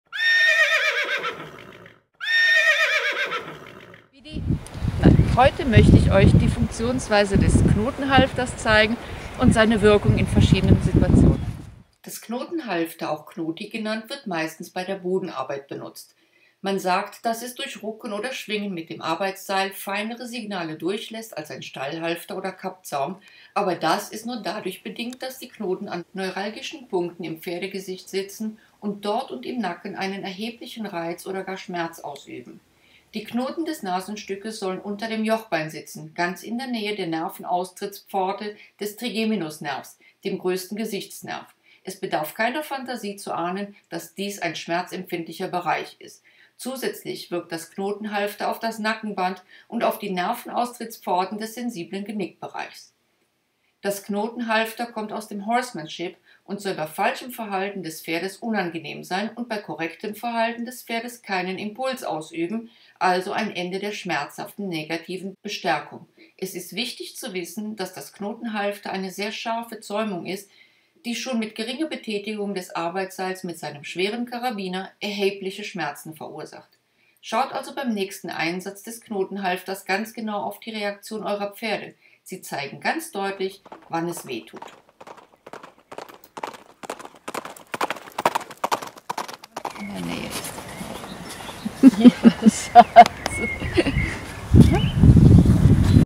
Das Knotenhalfter: Wirkungsweise, Einsatzbereiche und No-Go’s
Knotenhalfter, auch Konti genannt, wird meistens bei der Bodenarbeit benutzt.

Man sagt, daß es durch Rucken oder Schwingen mit dem Arbeitsseil, feinere Signale durchlässt als ein Stallhalfter oder Kappzaum, aber das ist nur dadurch bedingt, daß die Knoten an neuralgischen Punkten im Pferdegesicht sitzen und dort und im Nacken einen erheblichen Reiz oder gar Schmerz ausüben.

Die Knoten des Nasenstücks sollen unter dem Jochbein sitzen. Ganz in der Nähe der Nerveaustrittspforte des Trigeminusnervs, dem größten Gesichtsnerv. Es bedarf keiner Phantasie zu ahnen, daß dies ein schmerzempfindlicher Bereich ist. Zusätzlich wirkt das Knotenhalfter auf das Nackenband und auf die Nervenaustrittspforten des sensiblen Genickbereichs.

Das Knotenhalfter kommt aus dem Horsemanship und soll bei falschem Verhalten des Pferdes unangenehm sein und bei korrektem Verhalten des Pferdes keinen Impuls ausüben, also ein Ende der schmerzhaften negativen Bestärkung.Es wichtig zu wissen, daß das Knotenhalfter eine sehr scharfe Zäumung ist, die schon mit geringer Betätigung des Arbeitsseils mit seinem schweren Karabiner, erhebliche Schmerzen verursacht.

Schaut also beim nächsten Einsatz des Knotenhalfters ganz genau auf die Reaktionen eurer Pferde, sie zeigen ganz deutlich wann es weh tut.

Neben der Bodenarbeit, also den Übungen des Natural Horsemanships, Parelli & Co. eignet sich ein Knotenhalfter auch um mit dem Pferd spazieren zu gehen, denn: sollte es scheuen, kann man es mit einem kräftigen Impuls, der schmerzhaft ist, es leichter wieder zum „zuhören“ bringen.

Ein Knotenhalfter eignet sich nicht:
- Zum Anbinden, denn es hat keine Sollbruchstelle hat und die Pferde können sich beim Scheuen erheblich verletzen.
- Zum Longieren, weil es dabei verrutscht, die Knoten schmerzen verursachen können und eine reelle Stellung und damit Biegung nicht möglich sind.